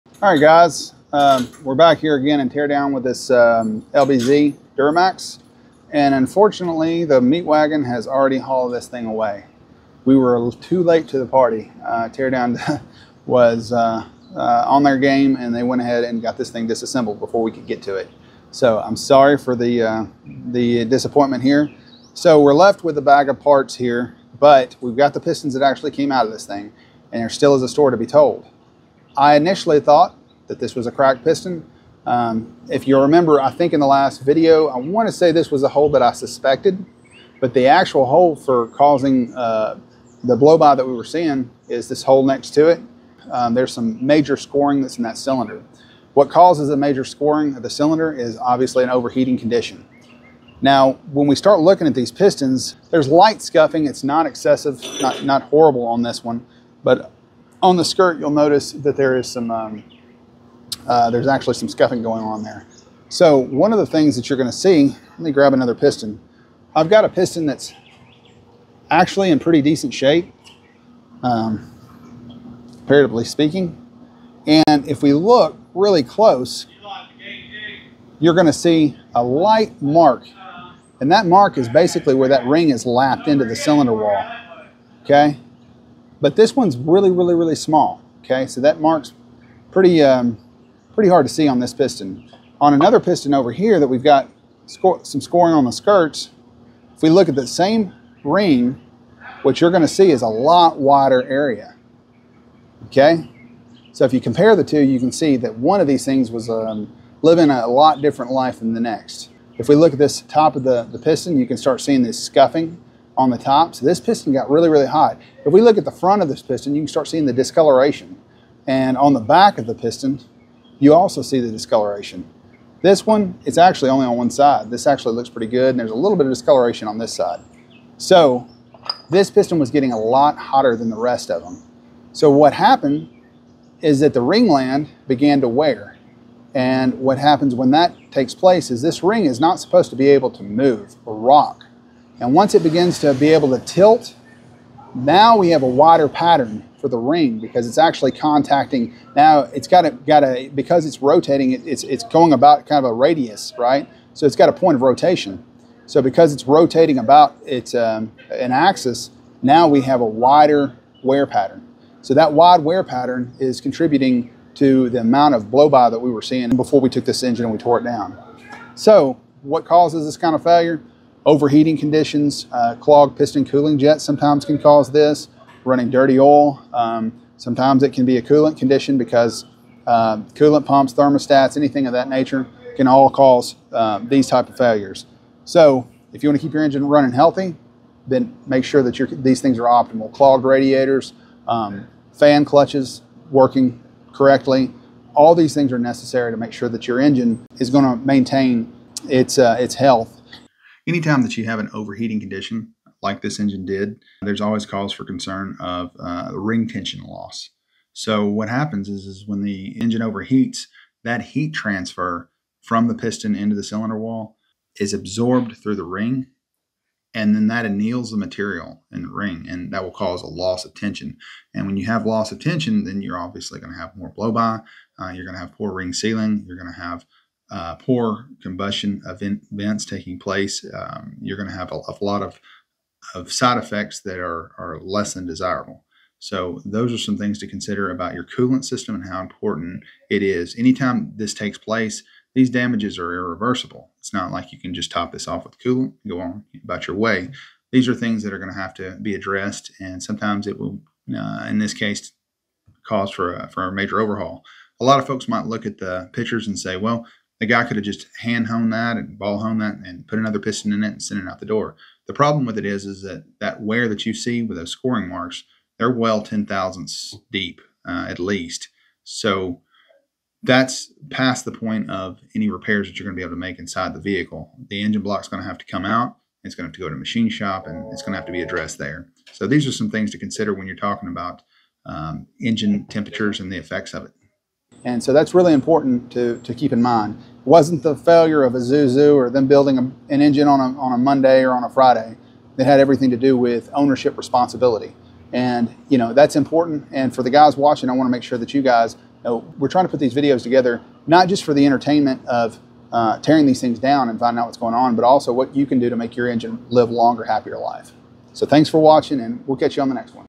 Guess the Failure Answer Revealed
Cass gives us the breakdown of what happened with this engine, in part two of "Guess the Failure."